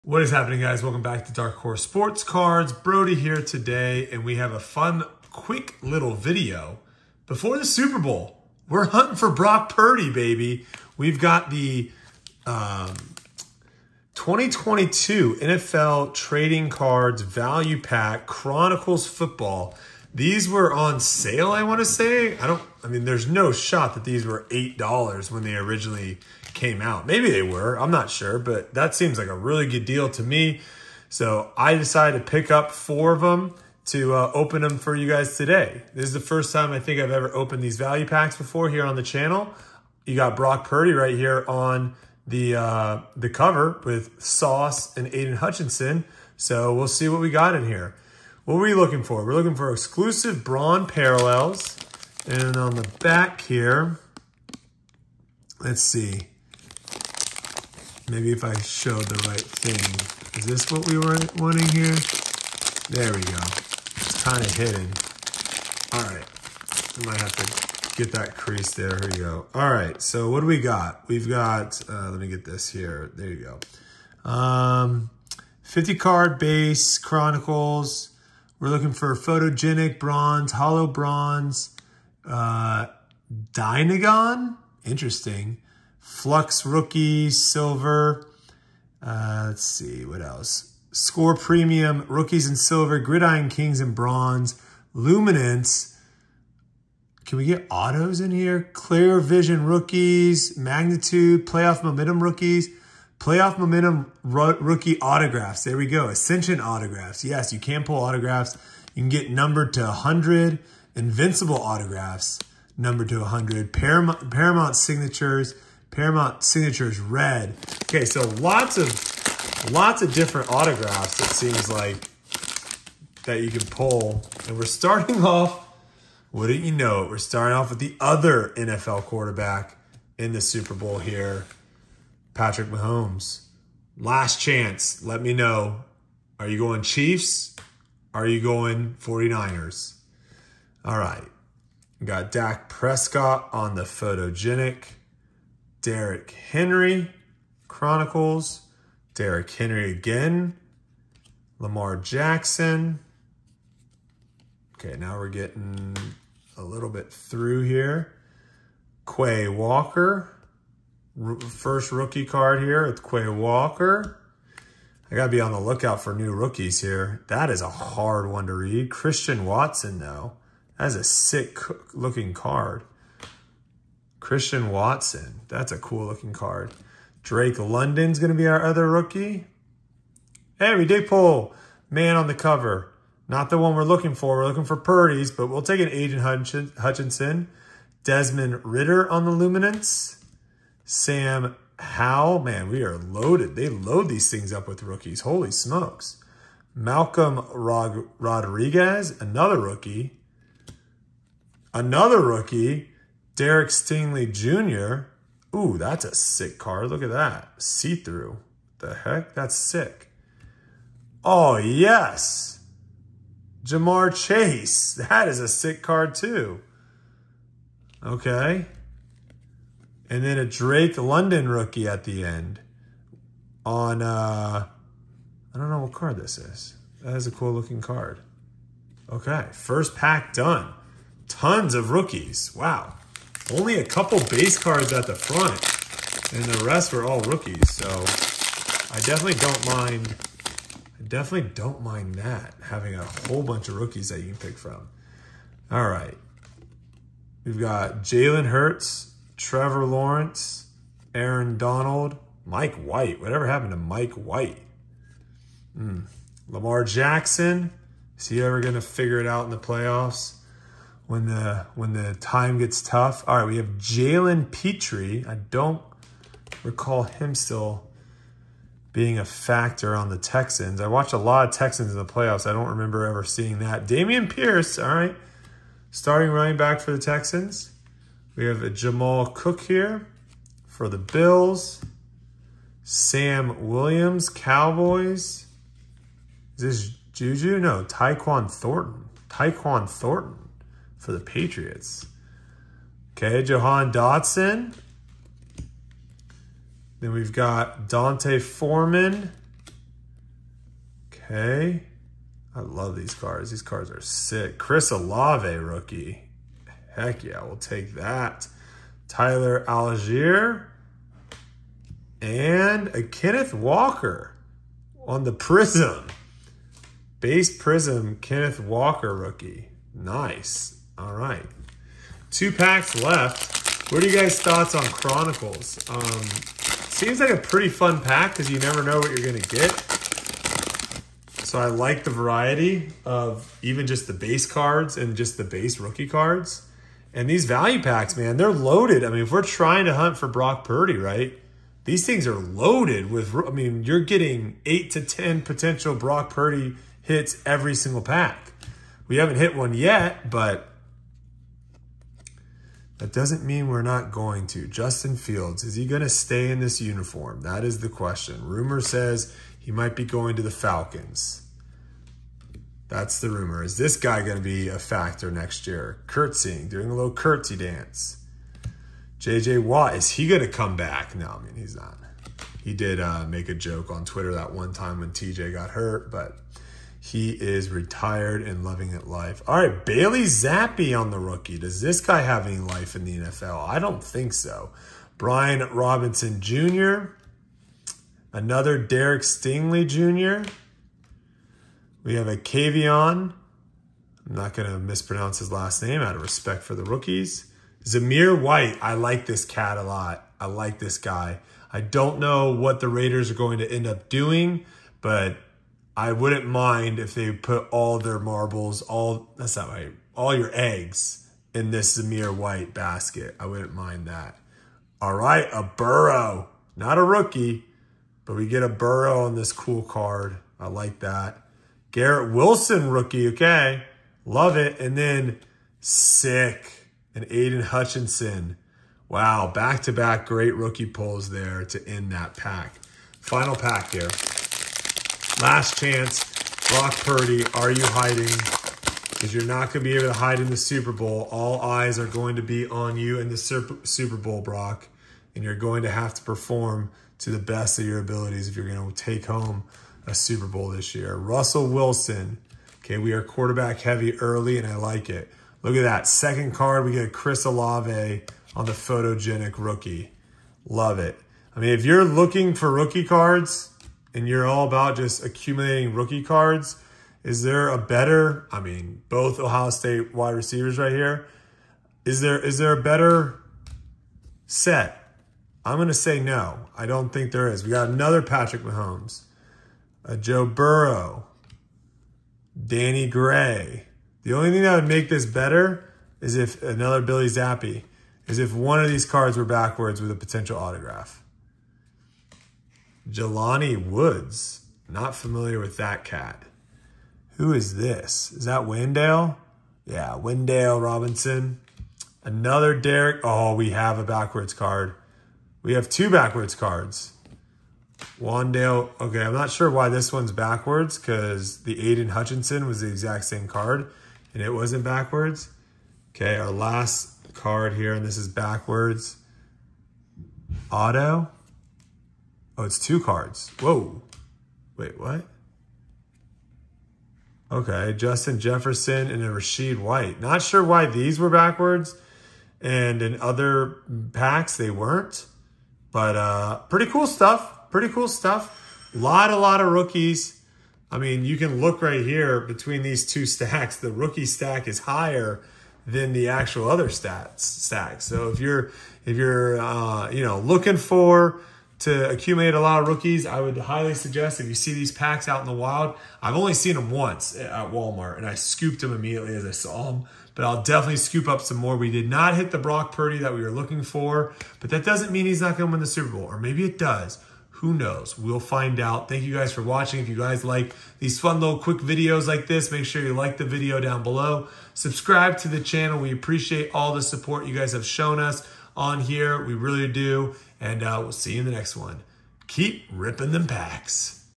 HUNTING FOR BROCK PURDY ROOKIE CARDS! 2022 CHRONICLES VALUE PACKS
Getting ready for the Super Bowl by hunting Brock Purdy rookie cards!

EBAY STORE

United States

We are opening a 2022 Chronicles Value Packs